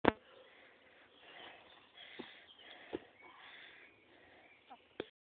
Mosiek salto Troszyn
Troche nisko, ale zrobiłem. komentujcie i dawajcie oceny.Nagrywane telefonem wiec moze byc niewyraźne.